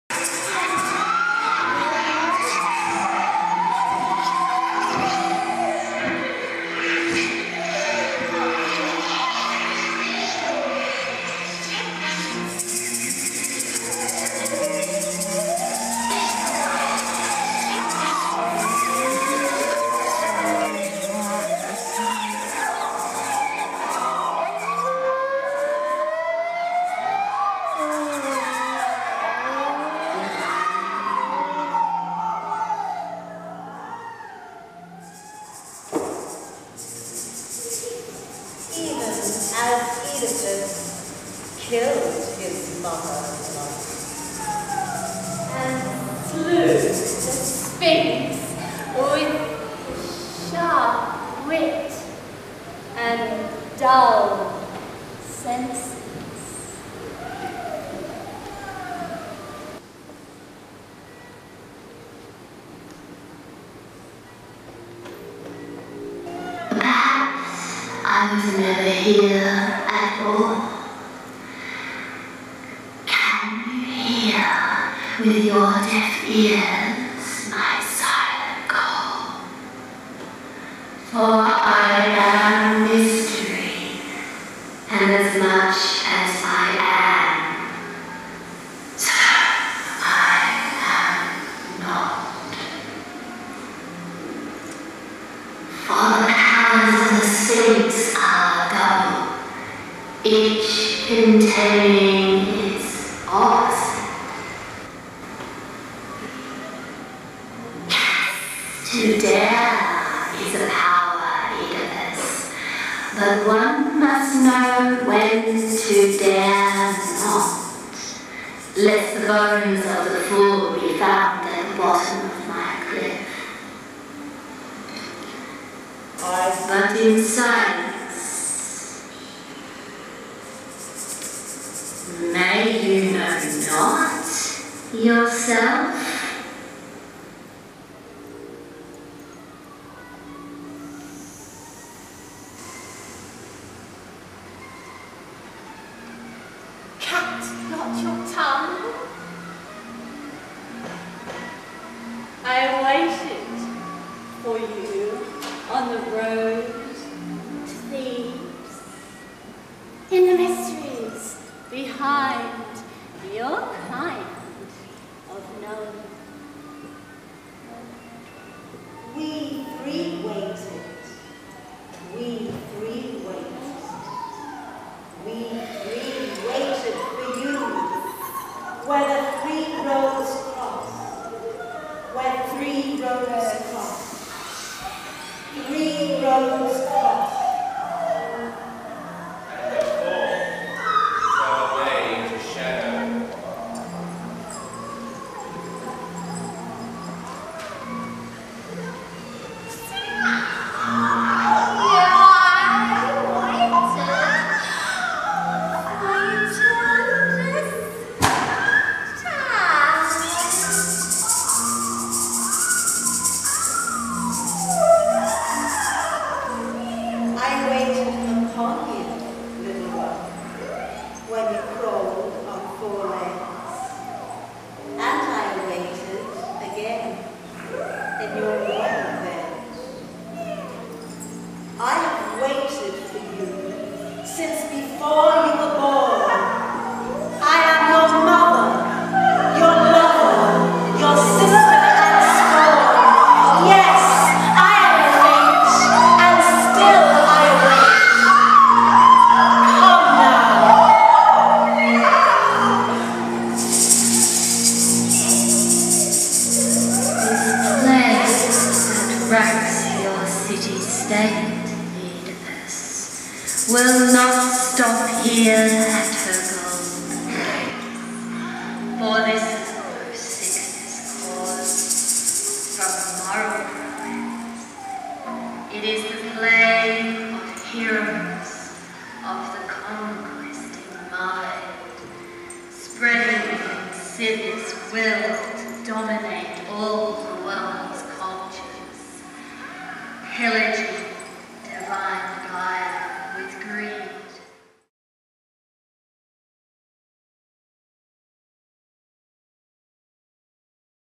An Extract from OEDIPUS TYRANNOS live in Melbourne March 09
OEDIPUS TYRANNOS is a reworking of the Ancient Greek Classic, demonstrating that Fate and free Will are NOT mutually exclusive. This is live footage from the Metamorphic Ritual Theatre performance in the Northcote Uniting Church, Melbourne Australia, 27th March 2009.  In this extract from the final scene, Oedipus' visionary epiphany reveals the Sphinx, Delphic Oracle and Jocasta as 3 Faces of his own deified Destiny, bringing his subconscious to light and revealing the true source of the plague which racks his city-state.
  Written by Orryelle with Giselle Sibyl. Oedipus-Orryelle, Sphinx-Giselle, Jocasta- Wendy Rule, Delphic Oracle/Pythia- Medea Luminea. Lighting by Jamee Sometimes. From footage by Melanie & Soncha Iachono.